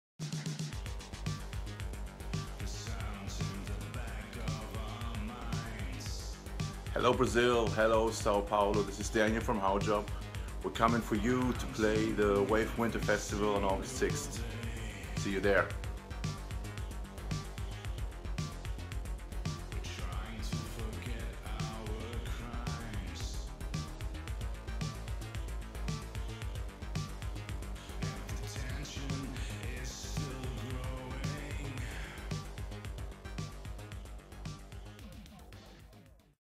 Convite (Daniel Myer) do HAUJOBB para Wave Winter Festival 2016
Convite de Daniel Myer (Haujobb) para assistirem o show do Haujobb no Wave Winter Festival que será dia 6 de agosto de 2016 a partir das 14 horas no Madame Club